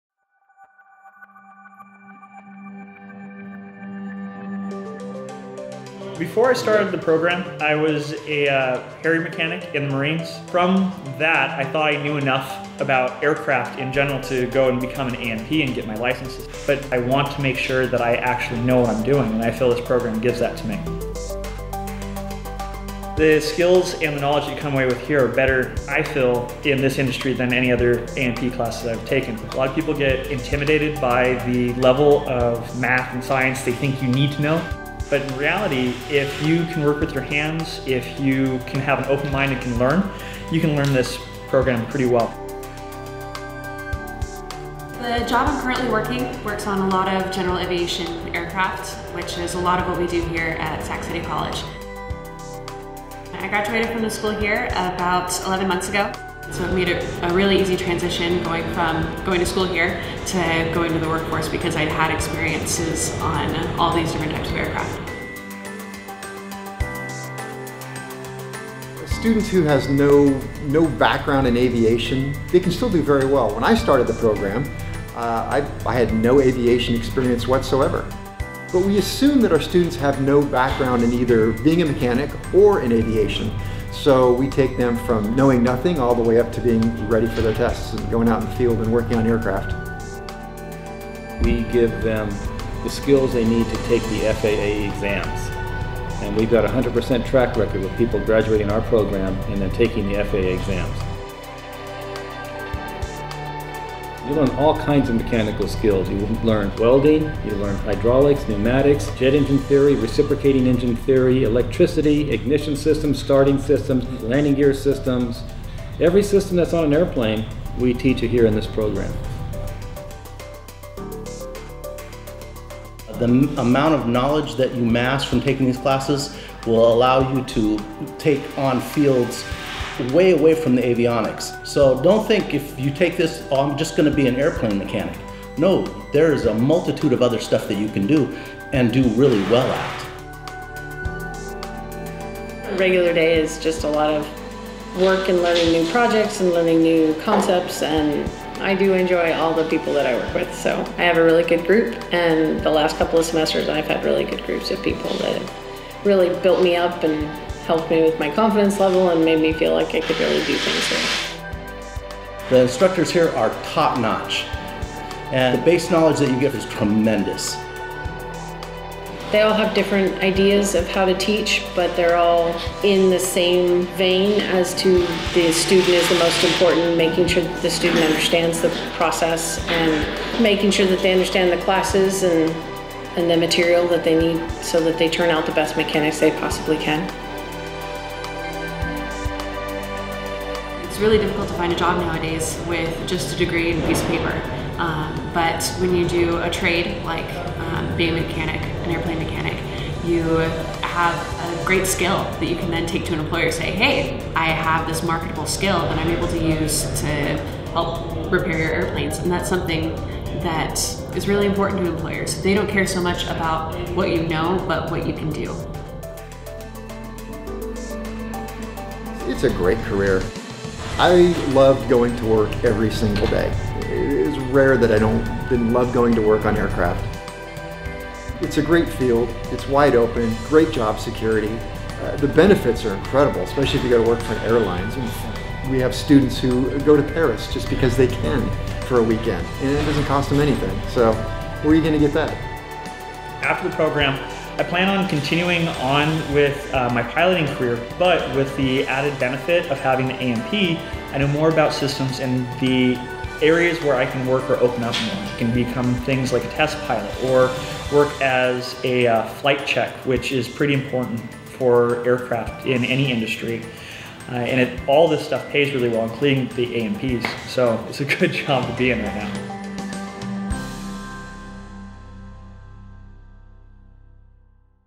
Sacramento City College Aviation Maintenance Program
Sacramento City College has one of the oldest Airframe and Powerplant programs in the world! Our graduates have a near 100% passing rate of the FAA exams.